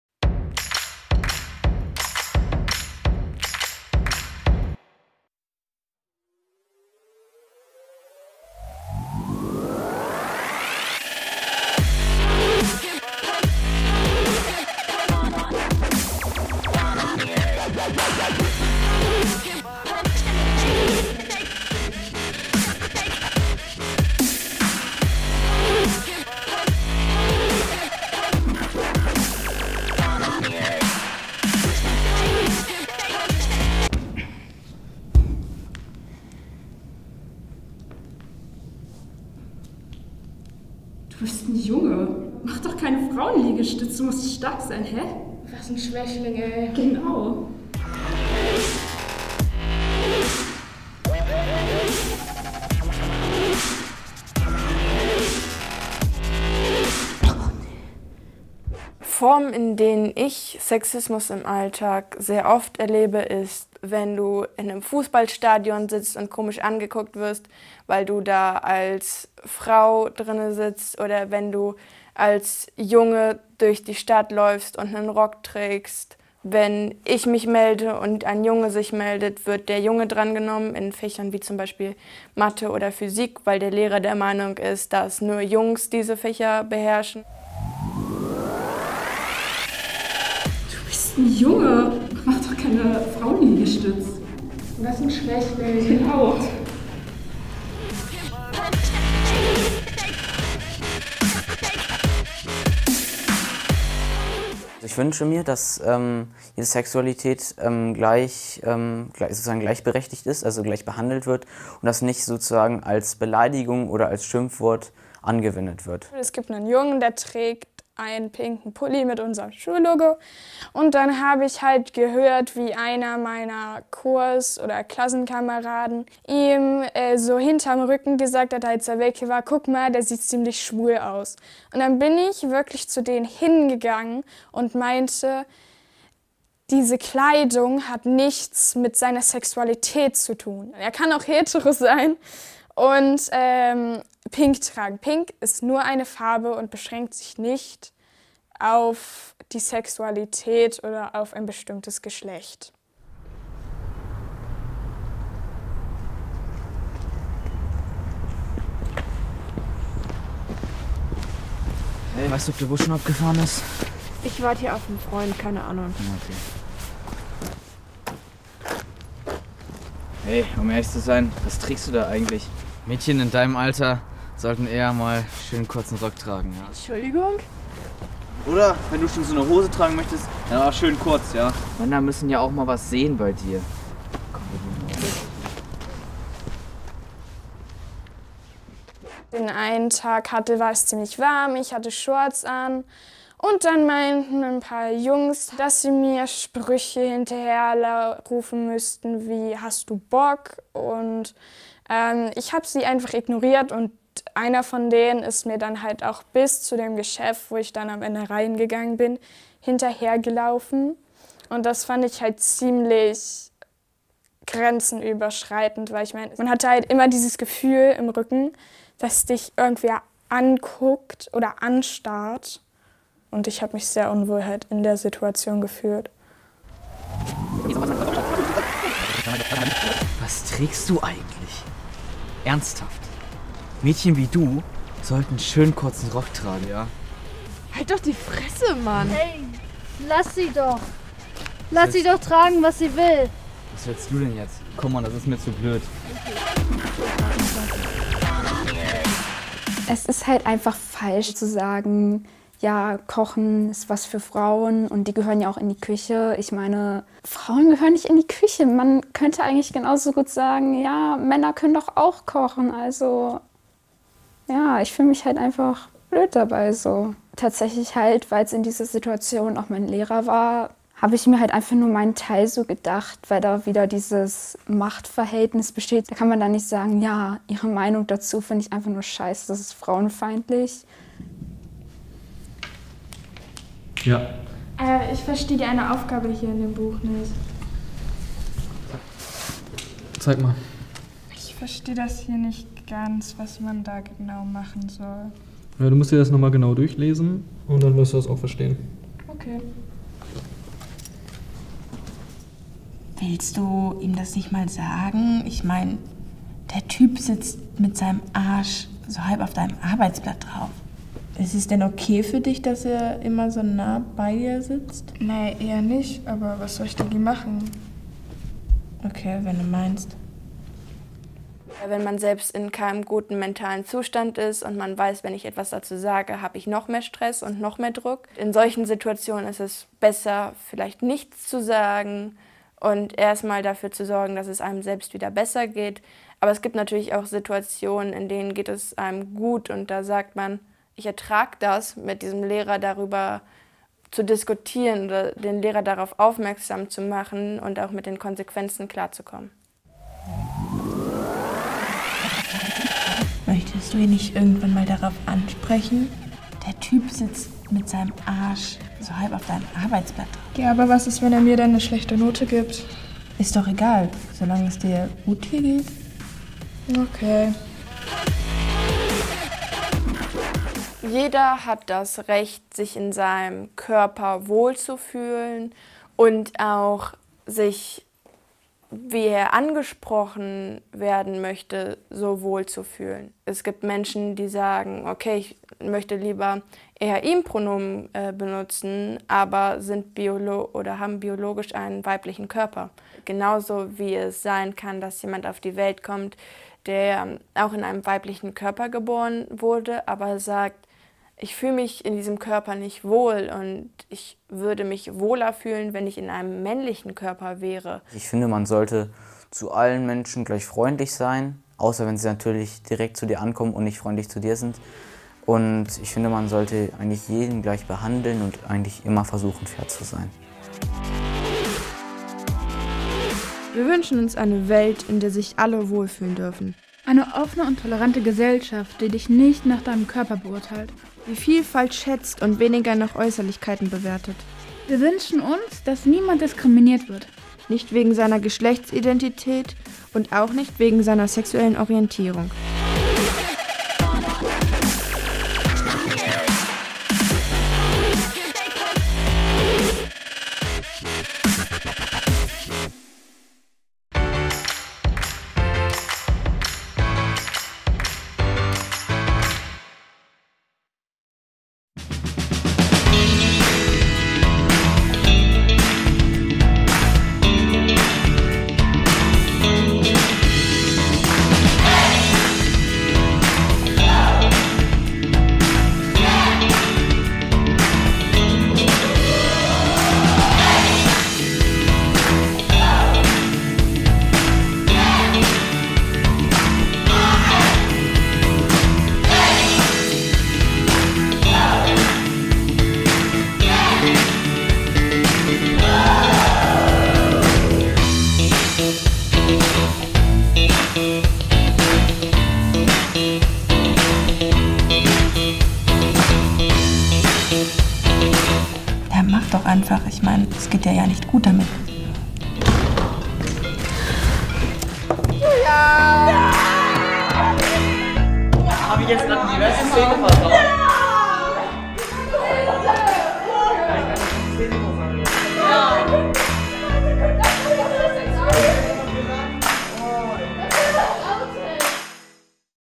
F*ck Sexism
Ob in der Schule, auf der Straße oder im Park: Im Alltag ist Sexismus nach wie vor ein großes Problem. Jungen und Mädchen werden ungleich behandelt, Menschen folgen toxischen Vorstellungen von Geschlechterrollen und werden belästigt oder greifen an. Dabei haben doch alle Menschen in ihrer breiten Diversität die gleichen Rechte?

UN-Kinderrechte | Artikel 2: Diskriminierungsverbot

Entstanden beim Projekt KLAPPE AUF! für Demokratie und Kinderrechte 2021.
Projektgruppe Jugendbeirat in Wolfsburg (Niedersachsen).

alleKINDER haben ein Recht darauf, gehört zu werden!